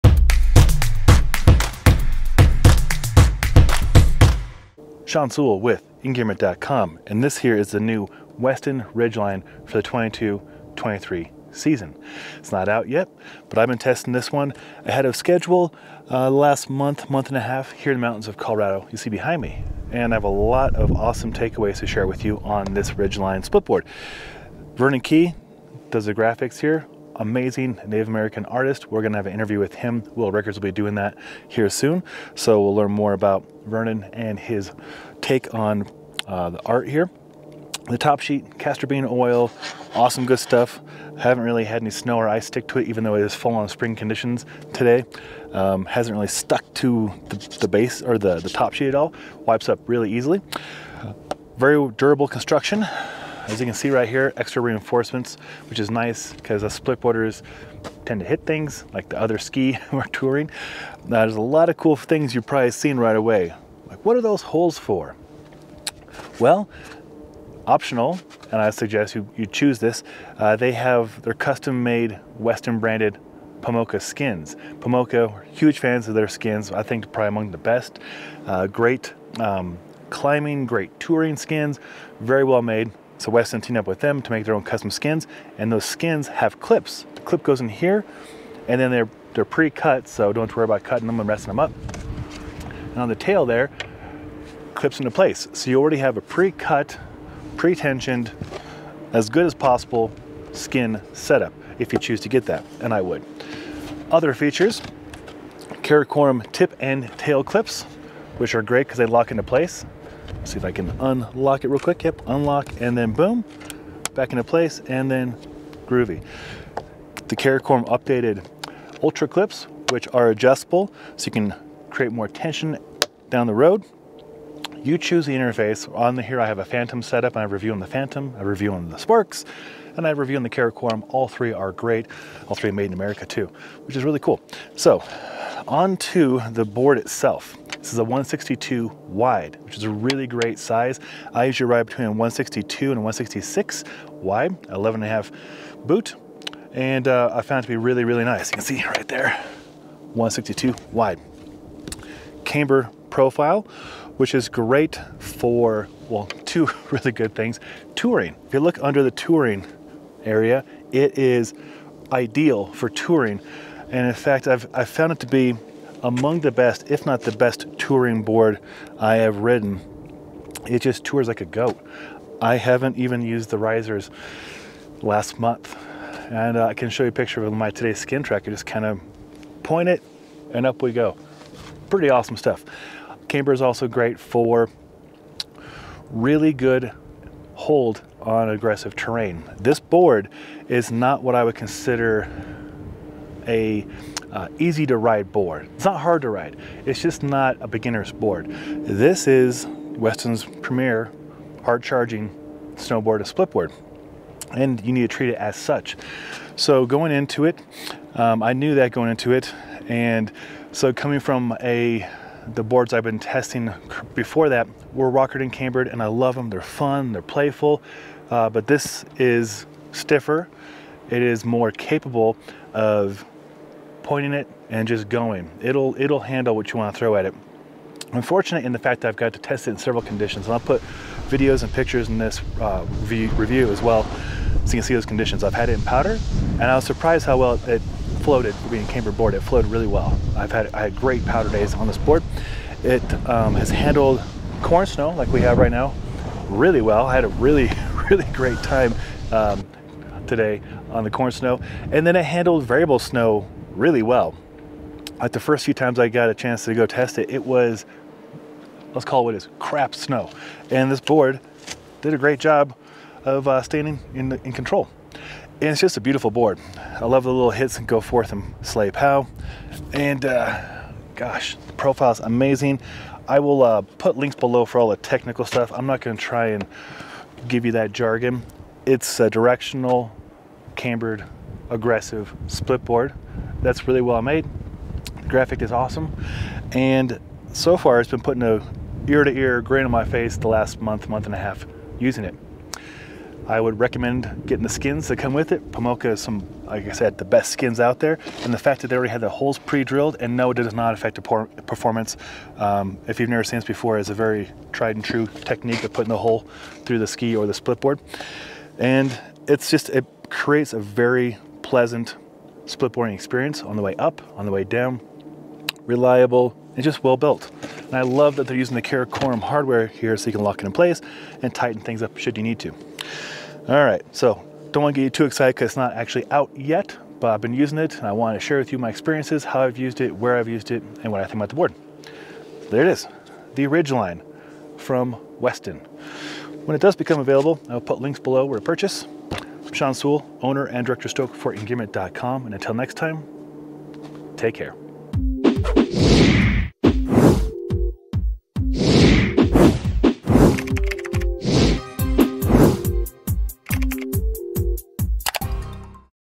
Weston Ridgeline Splitboard Review - New Art for 2022-2023
The new Weston Ridgeline Splitboard features an awesome new graphic from Native American artist Vernan Kee. We will have more info about him and his art in an upcoming interview.

The Ridgeline is meant to handle big lines and charge.  This is a stiffer splitboard and the camber profile is great for touring as well as keeping an edge. I used this in everything from powder to variable snow and even quite a bit of spring corn.

The new lineup from Weston also features optional pre-cut skins from Pomoca. The board has cutouts to work with the skins perfectly. Making touring even more streamlined.